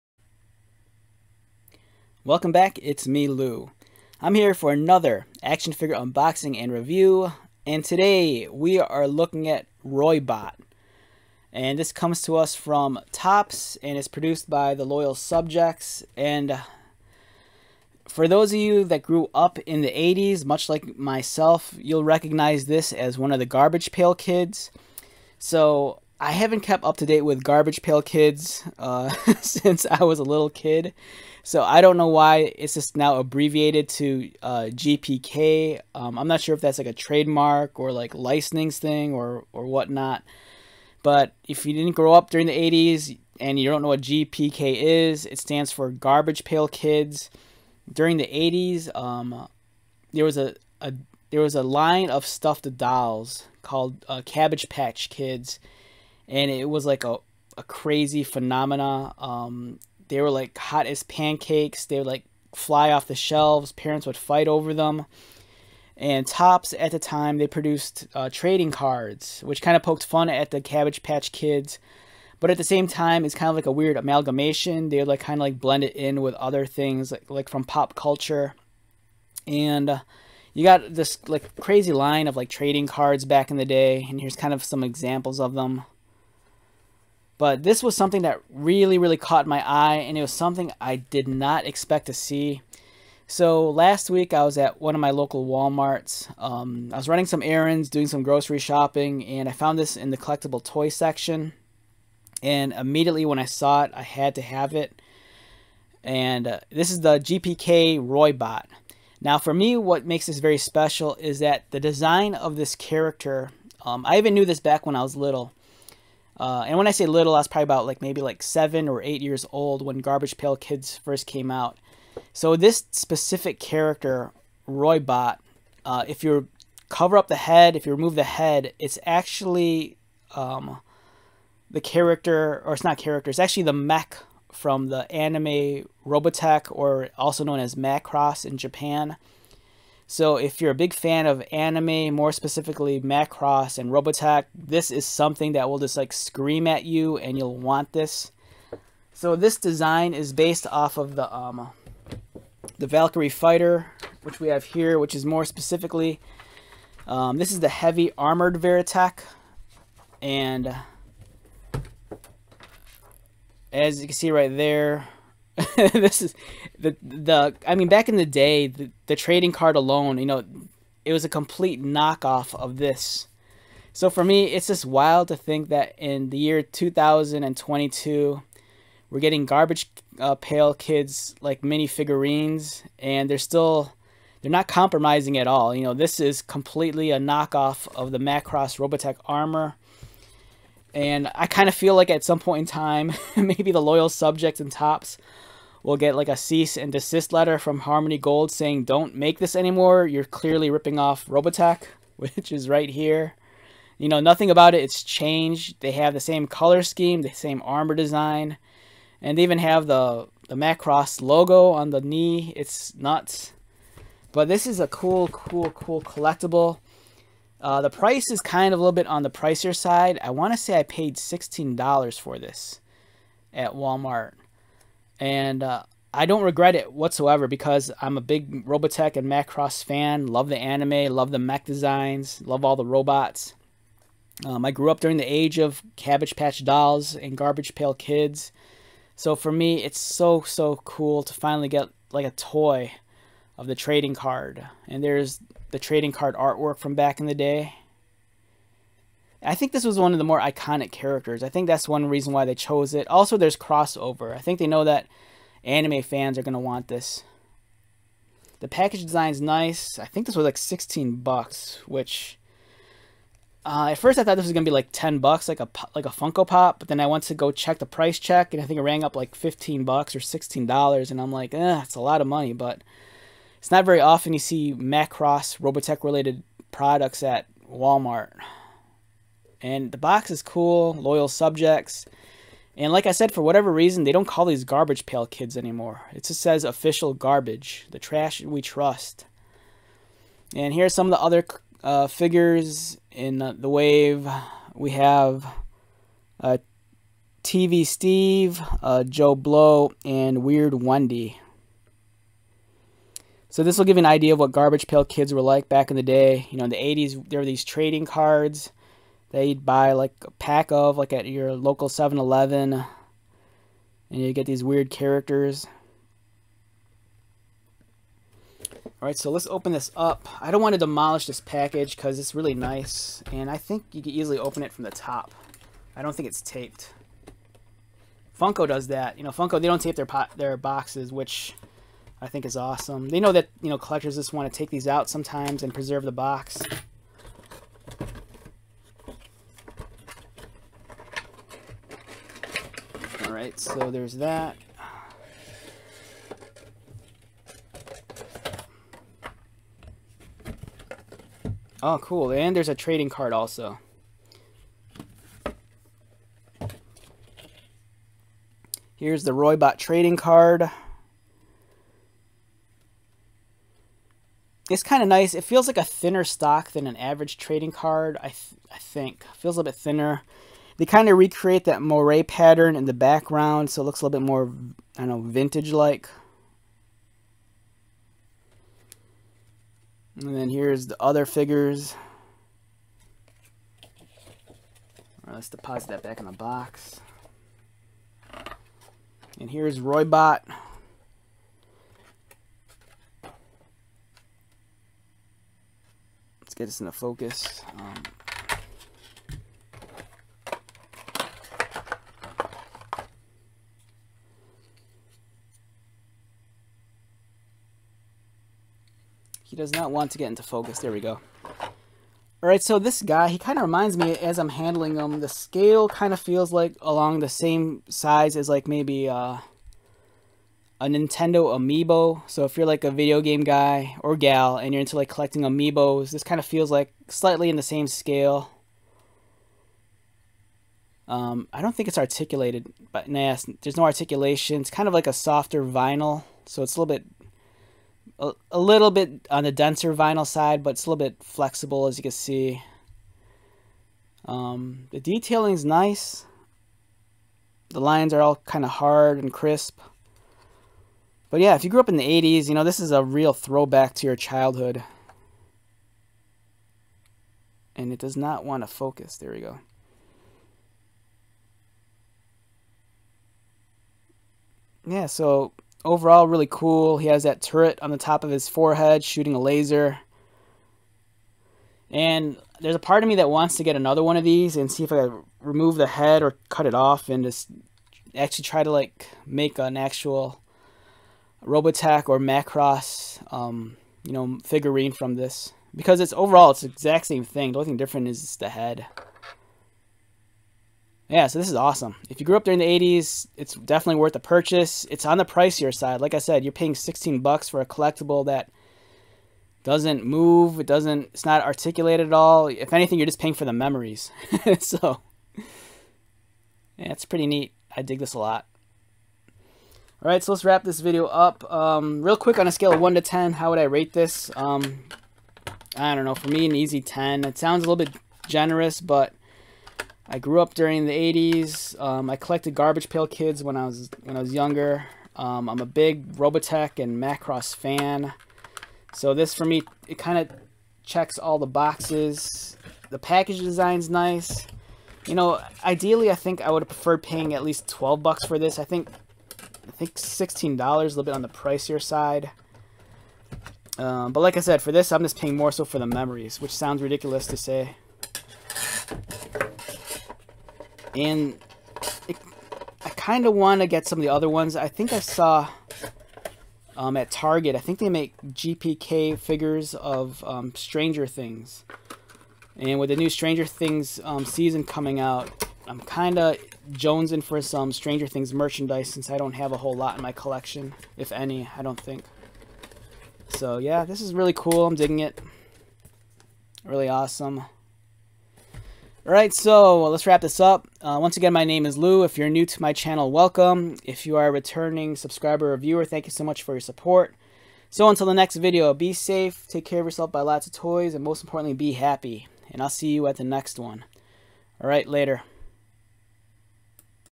Topps Loyal Subjects Garbage Pail Kids GPK Roy Bot Review
Welcome back! Today we are featuring Roy Bot.